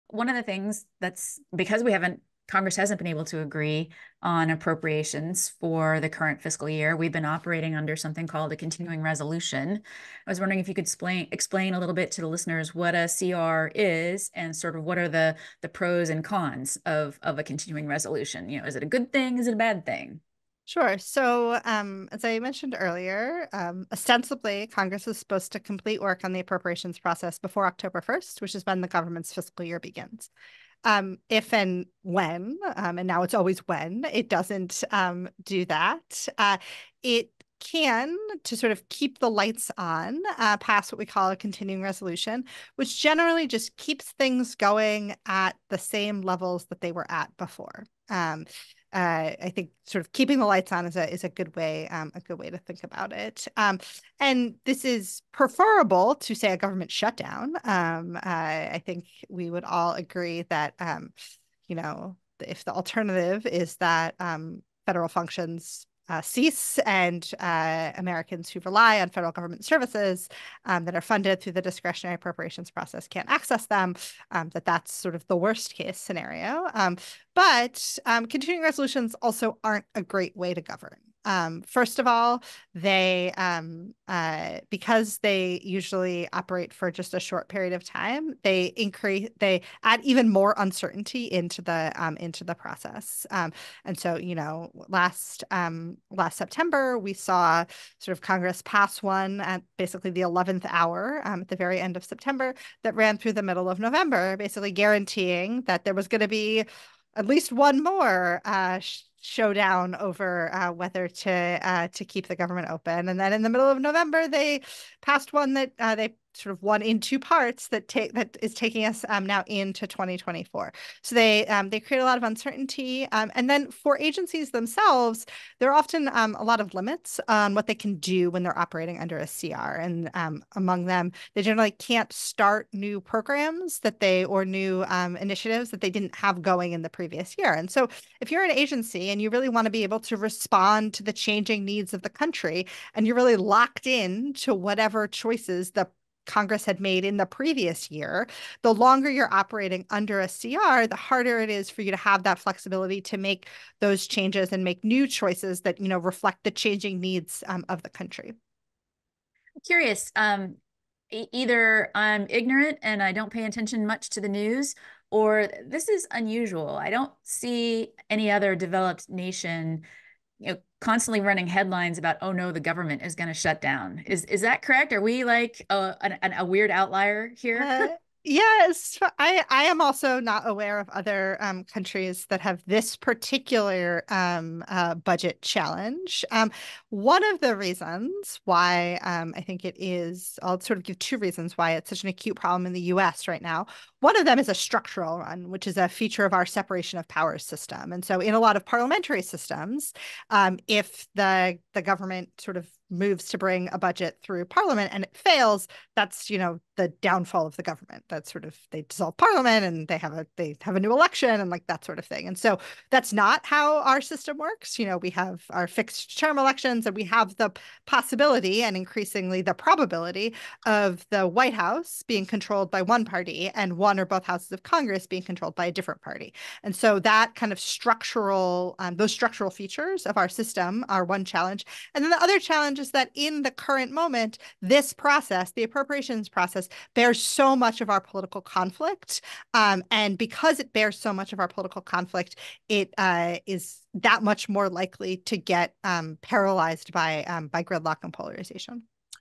The Problem with Continuing Resolutions...
This week on facingthefuture , we talked with Molly Reynolds, Senior Fellow in Governance Studies at the Brookings Institution, about the breakdown of the congressional budget process.

With another partial shutdown looming on January 19, we discussed why Congress never passes its annual appropriations bills on time, the problems this creates and whether there are any remedies.